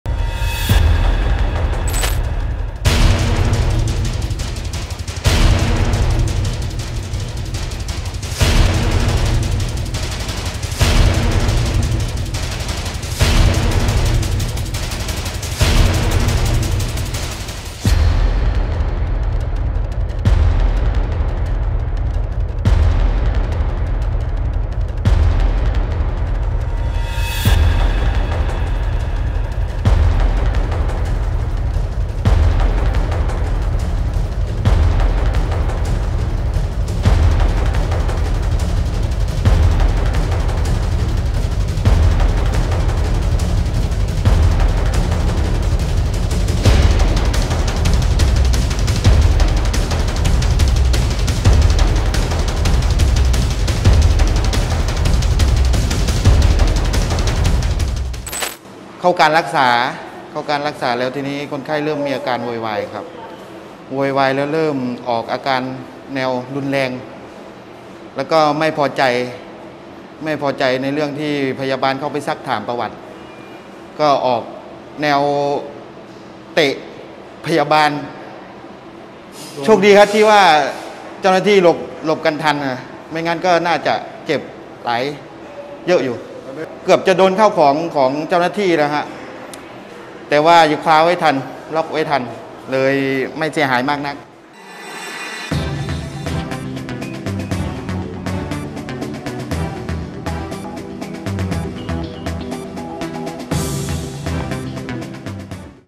ชายเมาหนัก ด่า-ทำร้ายเจ้าหน้าที่โรงพยาบาล | สำนักข่าววันนิวส์
หามชายหัวแตกส่งโรงพยาบาล เมาหนักอาละวาด ด่า-ทำร้ายเจ้าหน้าที่
หวิดเตะเสยคาง โชคดีหลบทัน กล่อมนานกว่า 10 นาทีจึงสงบ ล่าสุดโรงพยาบาลแจ้งความแล้ว